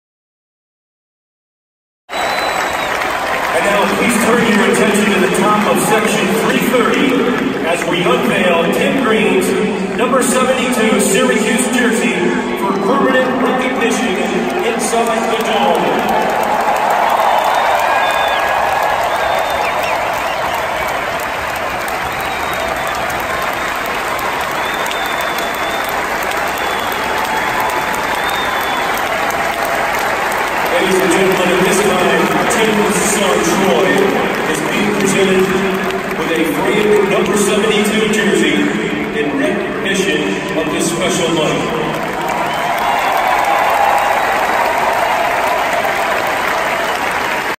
Syracuse football retires Tim Green’s jersey
Tim Green’s jersey No. 72 is retired during the Clemson game Sept. 14, 2019.